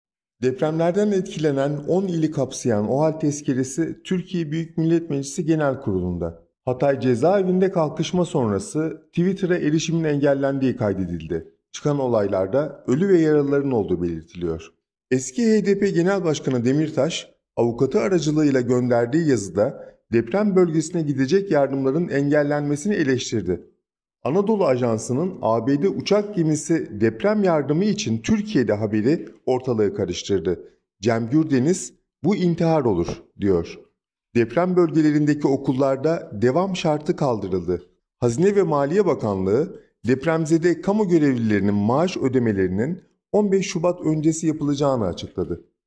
Hatay cezaevinde isyan çıktı, ABD uçak gemisi Türkiye'de | Toplumsal'ın Gündemi 13
OHAL tezkeresi TBMM'de

ABD’den bir garip destek!